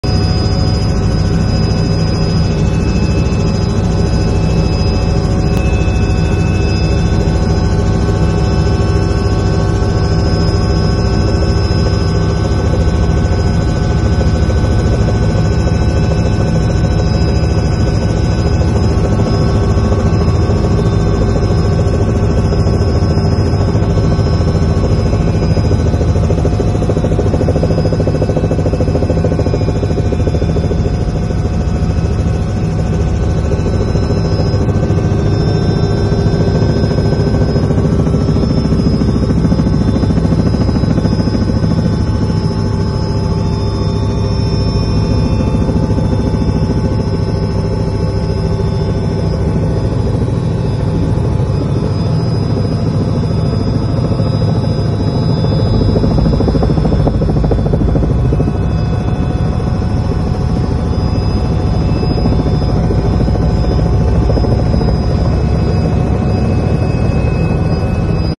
Горные Вертолеты. Ялта. Вертолет. СМУ.
горные вертолеты. Летим на Robinson R66 в сложных метео условиях. Крым, побережье черного моря!